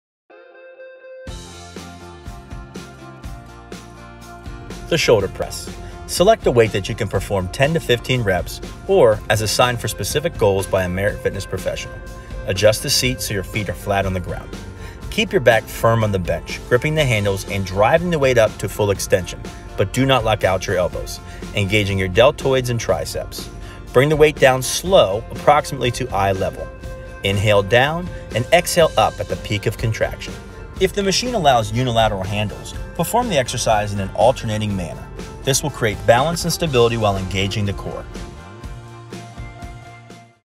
Shoulder Press Machine
Follow this guide for setting up the shoulder press machine and properly engaging your muscles.